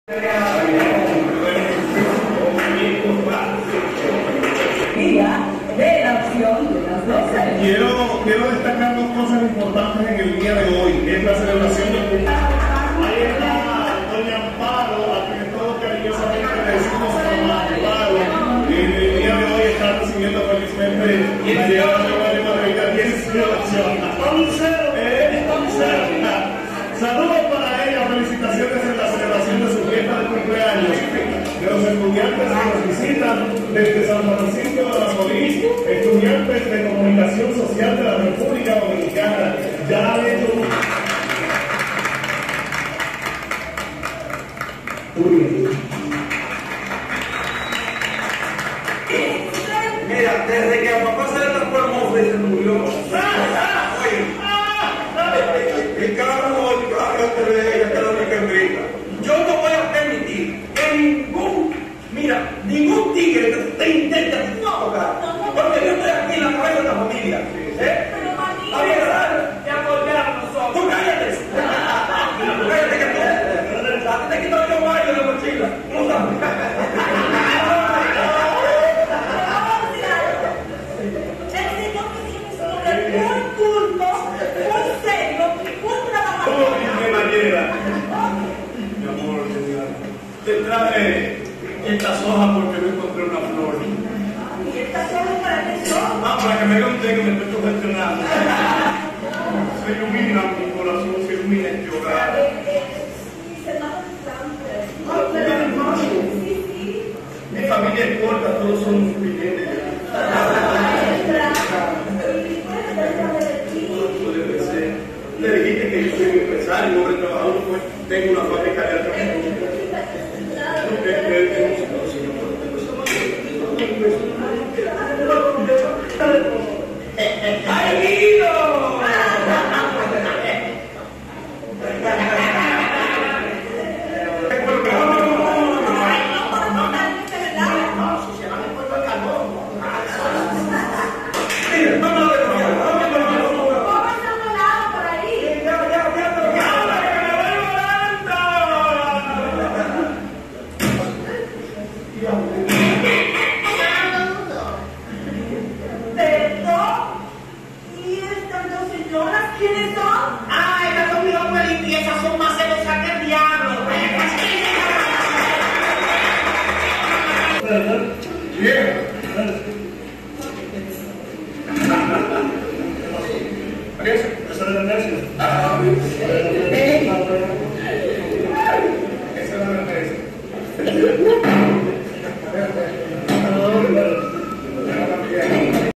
Tijuana TV R.D. Primera visita a Telemicro como estudiante de Comunicacion social año 2005.
Este contenido fué grabado detrás de camara por este servidor.